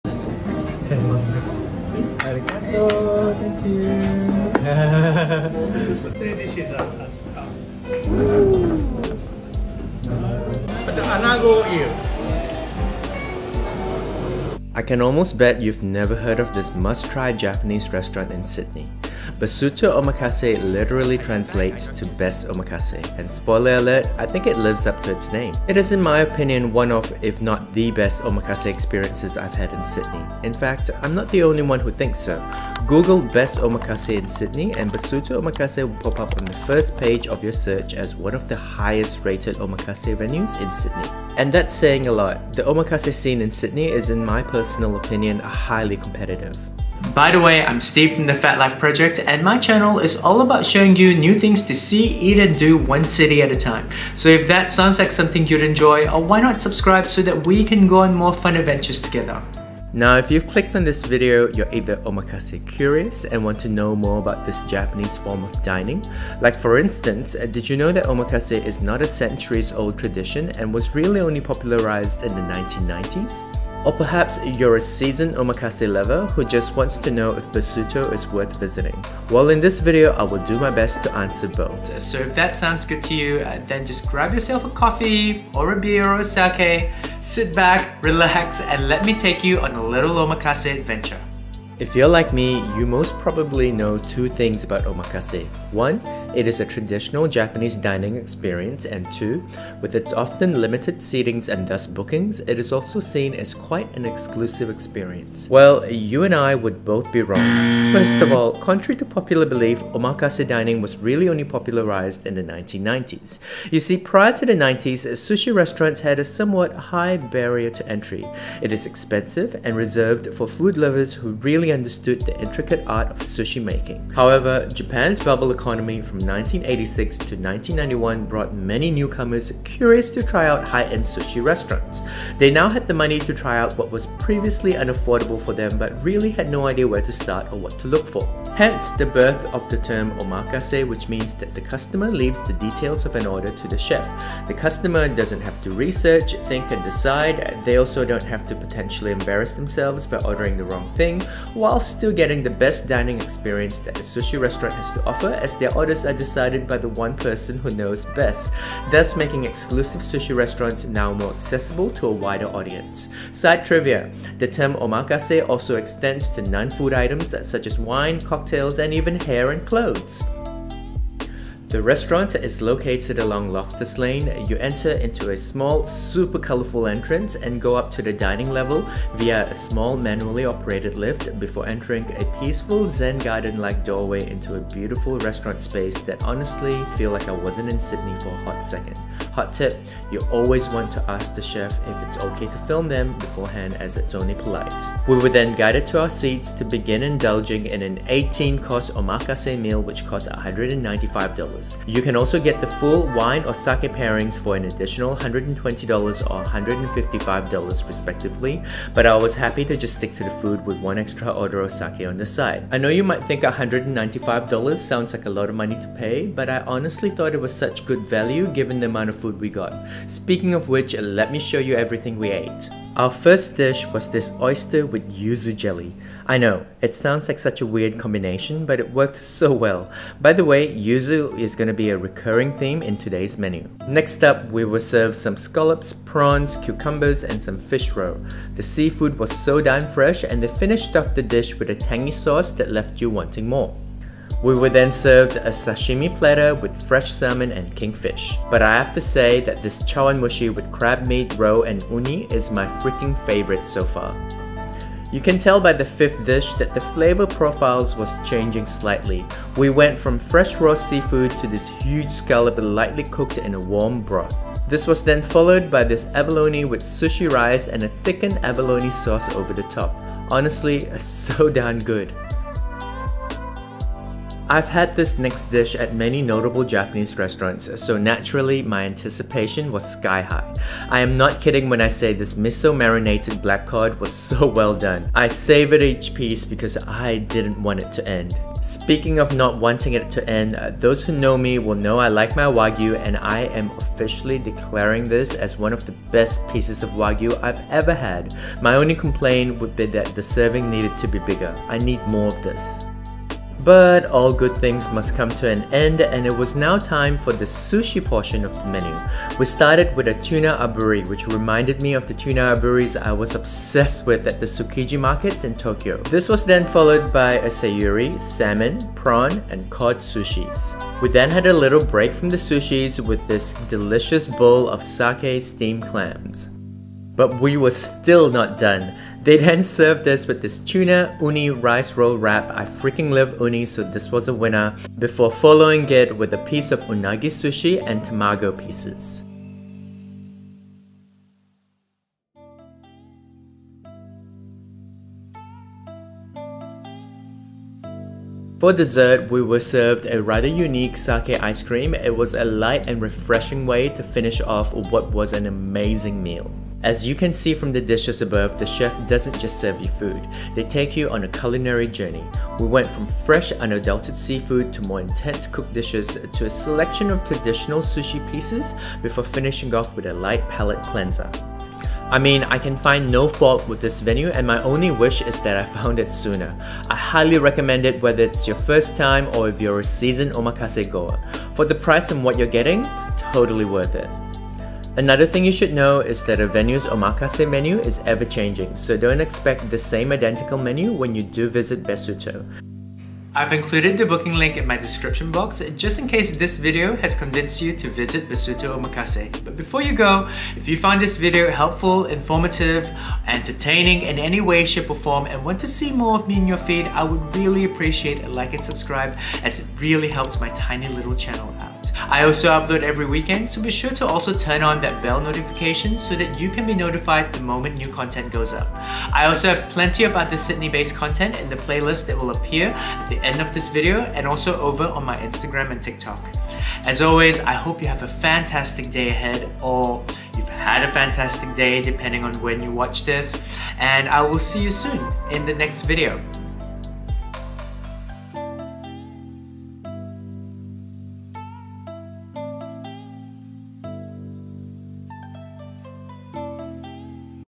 Must Try Sydney Restaurants | Besuto Omakase Review
I am literally still drooling thinking about Besuto Omakase, it is high on my must try sydney restaurants list

And I'm clearly not the only who thinks so, google best omakase in Sydney and this restaurant will appear right on the first page

In this video I will cover the origins of Omakase and review the eighteen course menu I had at one of sydney's best omakase restaurants (hands down)

Besuto Omakase, 6 Loftus Lane, Sydney

and would love to be friends with you there!

music Credits

00:00 Best Omakase in Sydney
01:31 Omakase Explained
02:52 Besuto Omakase
03:41 Besuto Omakase Menu Review
05:57 Overall thoughts
06:32 Besuto Omakase Bookings